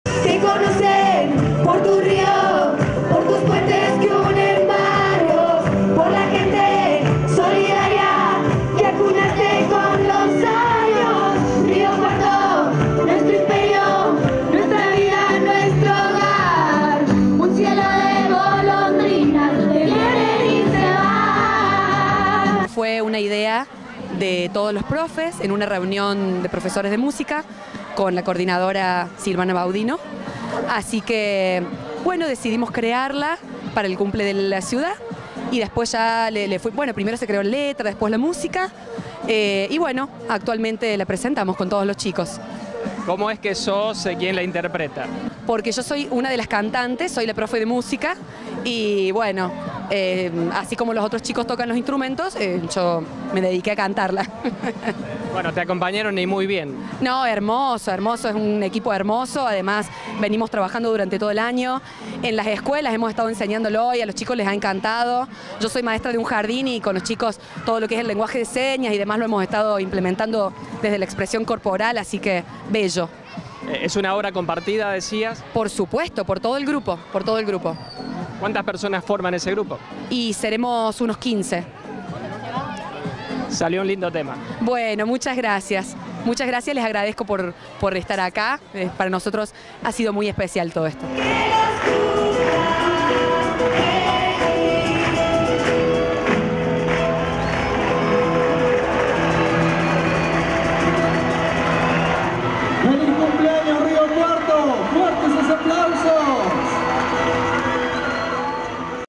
CUMPLEAÑOS DE RÍO CUARTO, PRESENTARON CANCIÓN
Con la presencia de 15 colegios se desarrolló hoy un emotivo acto frente al municipio, donde se presentó la canción dedicada a nuestra ciudad. La obra fue escrita y musicalizada por docentes de música de colegios de la cuarta región escolar.